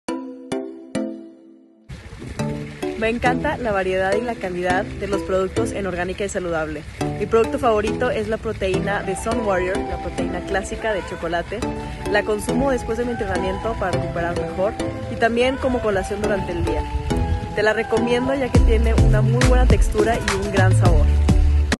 Natalia Gómez Junco, atleta y fan de Orgánica y Saludable
Atletas profesionales incluyen nuestros productos en su día a día, una de ellas es Natalia Gómez Junco, futbolista mexicana que te cuenta cuál es su producto favorito. ✨@ngomezjunco